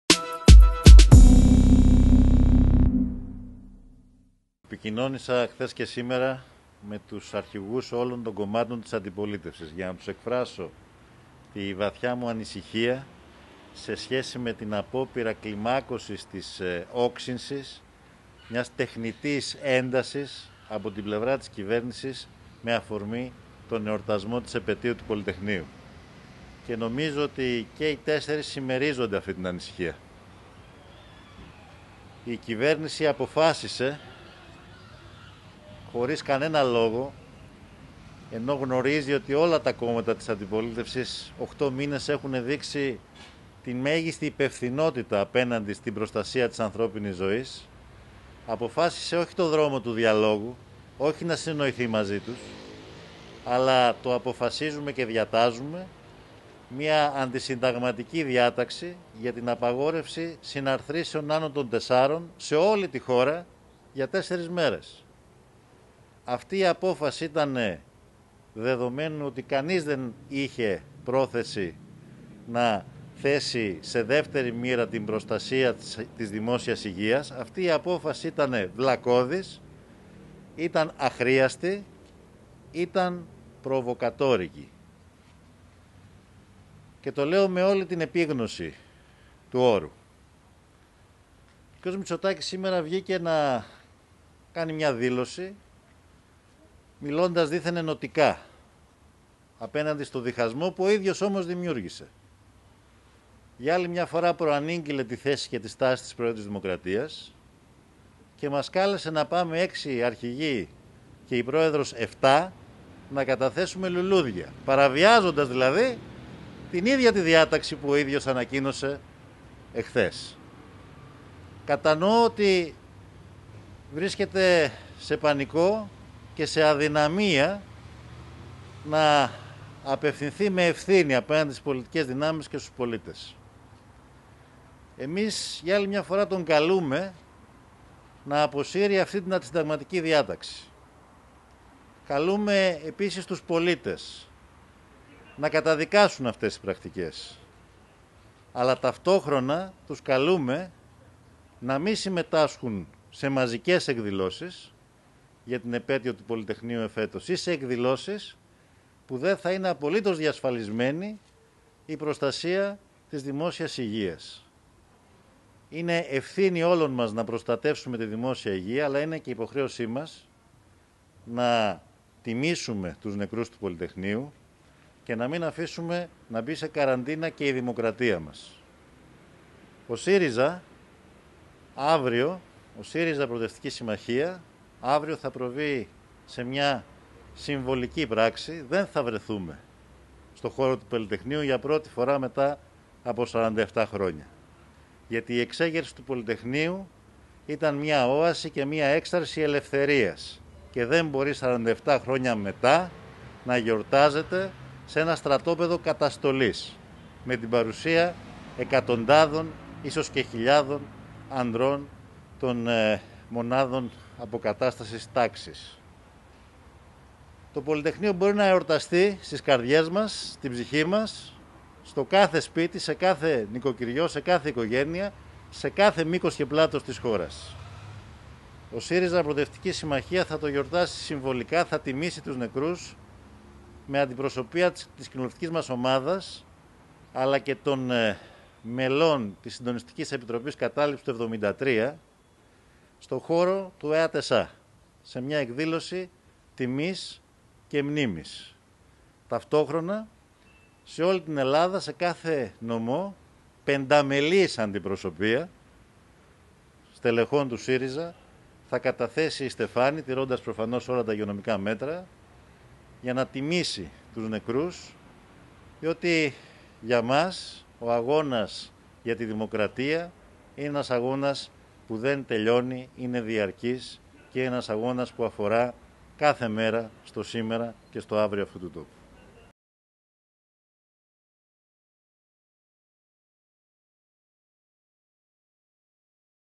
ΣΥΡΙΖΑ: Κρεσέντο υποκρισίας. Ο φαρισαϊσμός του κ. Μητσοτάκη ξεπερνά πλέον κάθε φαντασία.
«Ο κ. Μητσοτάκης σε ένα κρεσέντο υποκρισίας και αφού έχει αναστείλει το Σύνταγμα με απαγόρευση συναθροίσεων πανελλαδικά και έχει απειλήσει κόμματα και φορείς με συλλήψεις αν τιμήσουν συμβολικά τη μνήμη της επετείου του Πολυτεχνείου, μιλάει τώρα εναντίον του διχασμού. Του διχασμού που ο ίδιος δημιούργησε» ανέφερε απαντώντας στις δηλώσεις του πρωθυπουργού ο Πρόεδρος του ΣΥΡΙΖΑ, Αλέξης Τσίπρα έξω από το Περιστύλιο της Βουλής, τη Δευτέρα 16 Νοεμβρίου 2020.
Προσθέτει ότι ο κ. Μητσοτάκης «για ακόμη μία φορά προαναγγέλλει, και μάλιστα δημόσια, τη στάση και τη θέση της Προέδρου της Δημοκρατίας, μη αντιλαμβανόμενος ότι ο θεσμός του Αρχηγού του κράτους δεν είναι επικουρικός στο ρόλο της κυβέρνησης».
«Ωστόσο», συνεχίζει, «η πρότασή του για τη συμμετοχή των έξι πολιτικών αρχηγών από κοινού με την Πρόεδρο της Δημοκρατίας προσκρούει στην ίδια διάταξη δημοσίων συναθροίσεων άνω των 4 ατόμων, την οποία η κυβέρνησή του ανακοίνωσε», για να σχολιάσει ότι «ο φαρισαϊσμός του κ. Μητσοτάκη ξεπερνά πλέον κάθε φαντασία». ΕΡΤ / ΑΠΕ ΜΠΕ